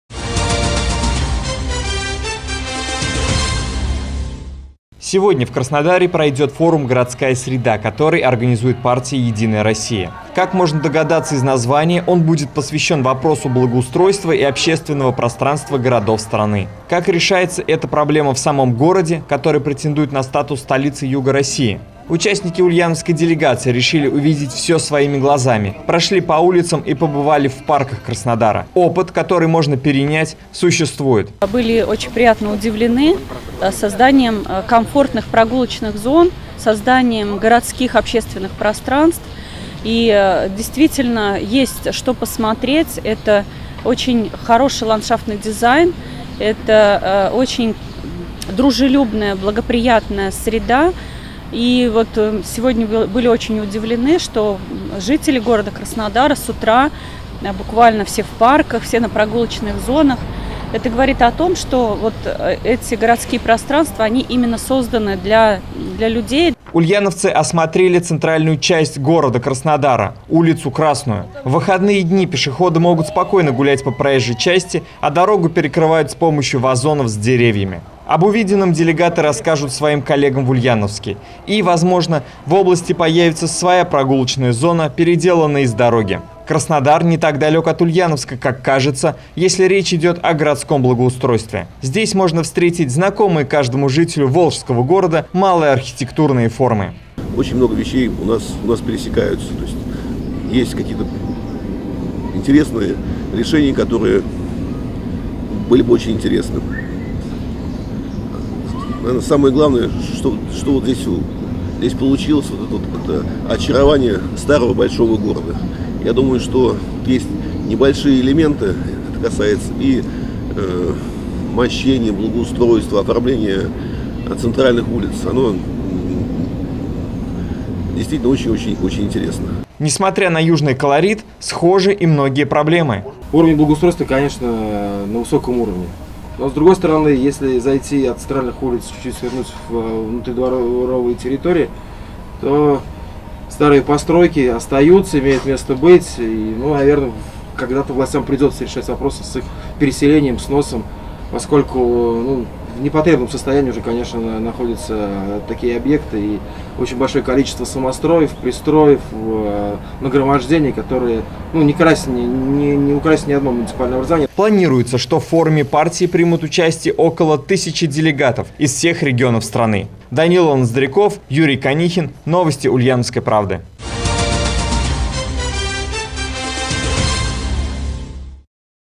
Ульяновцы научились в Краснодаре, как сделать улицы пешеходными
Cегодня в Краснодаре пройдёт форум «Городская среда», который организует партия «Единая Россия». Как можно догадаться из названия, он будет посвящён вопросам благоустройства и общественным пространствам городов страны.